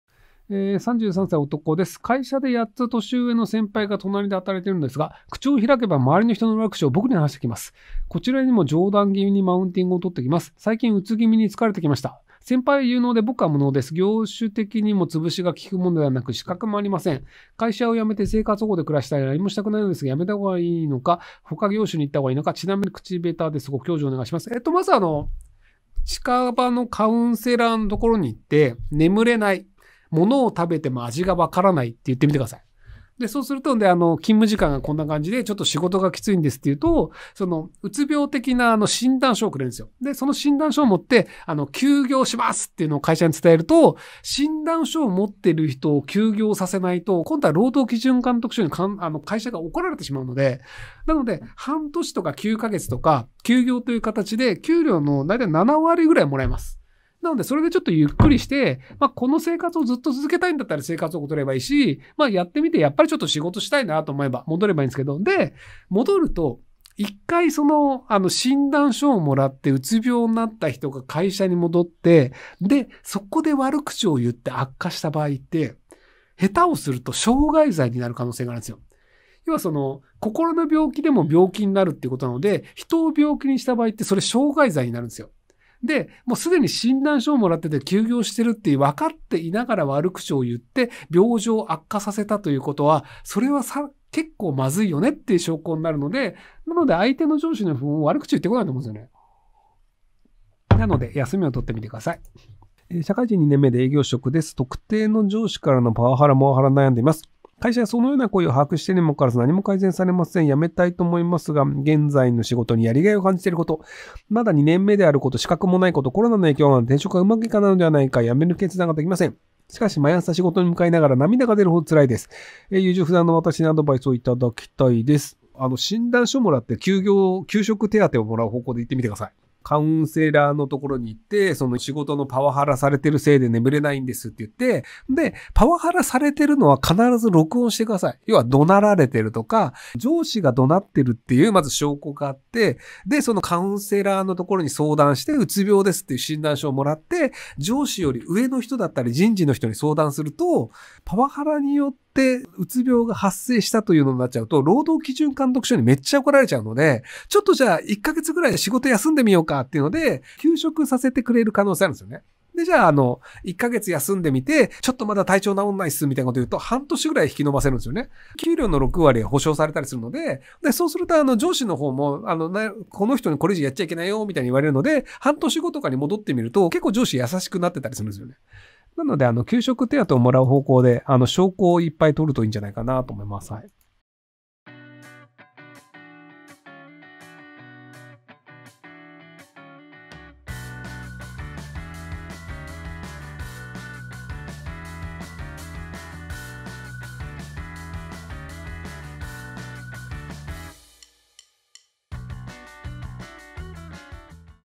【ひろゆき】うつ病なら転職するよりも休職した方がお得！上手な休職方法も解説【切り抜き/論破】
うつ病ならすぐ転職するよりも休職した方がお得！上手な休職方法もひろゆきさんが分かり易く解説します！
周りにうつ病で休職した後に復職した人がいる場合の注意点も解説！

ひろゆきさんの雑談生放送の見どころや役立つ情報を編集し、類似内容をまとめた上で切り抜き動画を投稿しています。